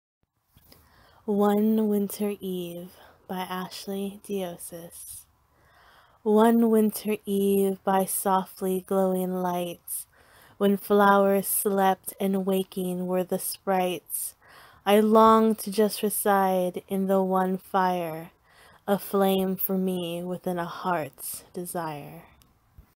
One Winter Eve by Ashley Dioses
One Winter Eve by Ashley Dioses.  This poem was first published in her debut collection, Diary of a Sorceress.